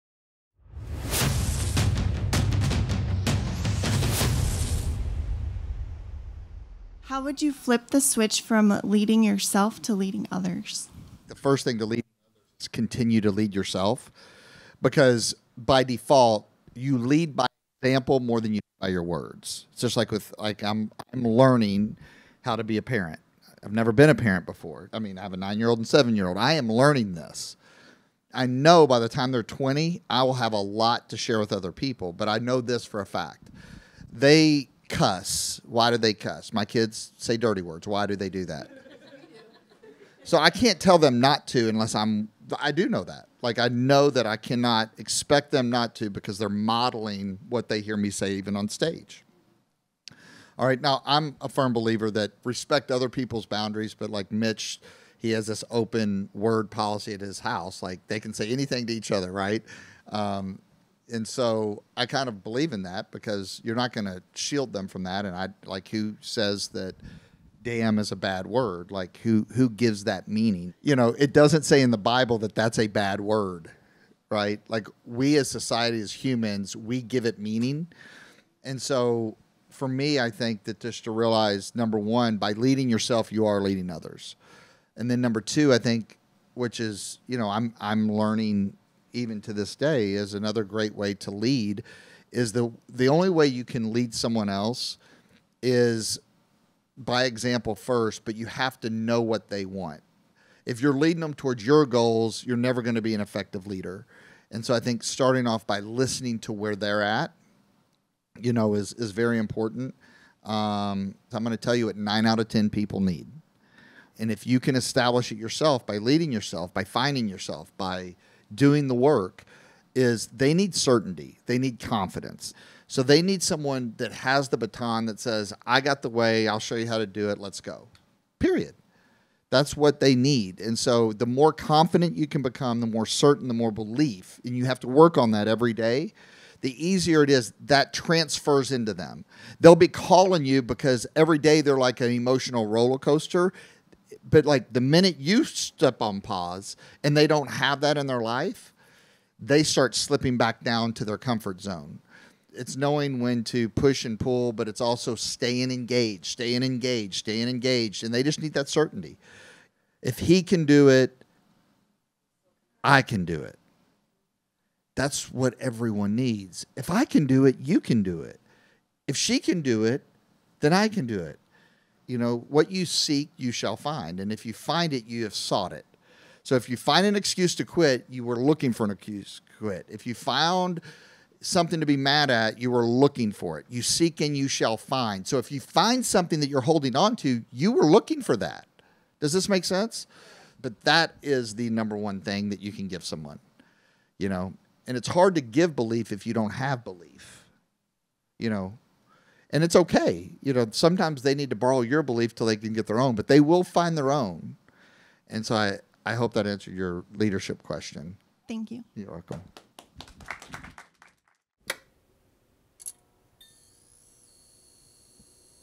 #095 Making The Switch From Leading Yourself To Leading Others
In a Q&A, I was asked how you switch from leading yourself to leading others. This episode is my 3-part approach to making that transition successful.